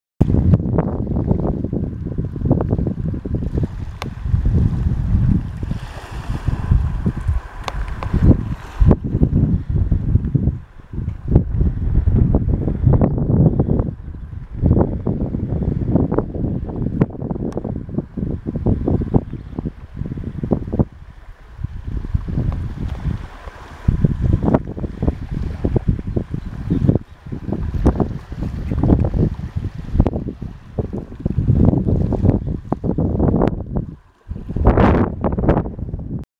Fur - Northern Coast
A part of the northern coastline of the island of Fur in Denmark. The red cliff in the end is Den Røde Sten (The red rock), which gets its color from rusted iron in the rock.